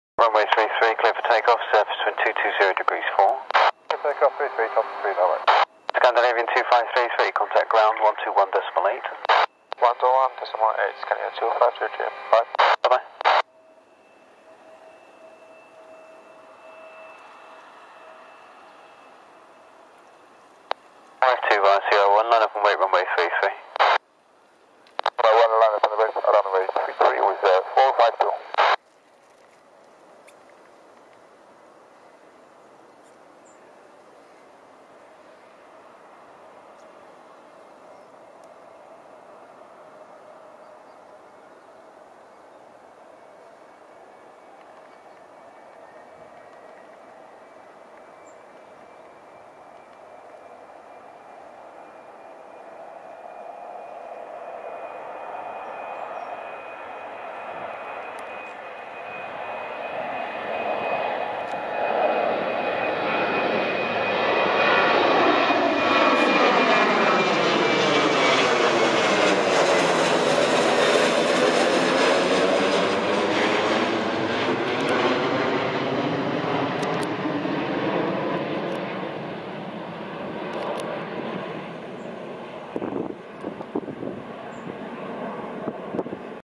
Thomson Airways B737 - Birmingham To Palma De Mallorca
Birmingham Airport
TOM7688 - Birmingham To Palma De Mallorca
Thomson Airways B737
Registration: G-FDZT
Takeoff: Runway 33